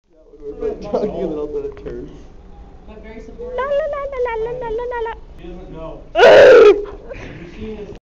Tennis Ball Part 2
Thought he was takin a walk alone but was wrong.. Bad news for the camera guy!